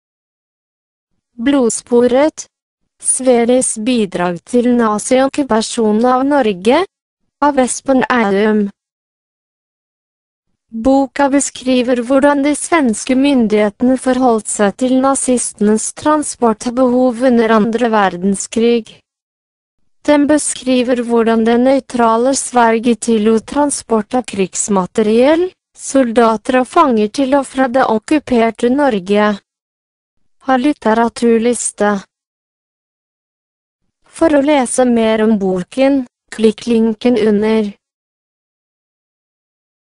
Blodsporet - Espen Eidum
Blodsporet : Sveriges bidrag til nazi-okkupasjonen av Norge. Av Espen Eidum. Boka beskriver hvordan de svenske myndi...